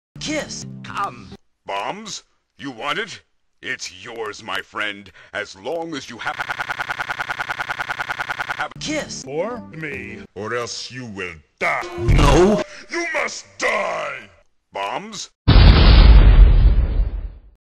BombGuy Strikes again
He wnats a kiss but mario says no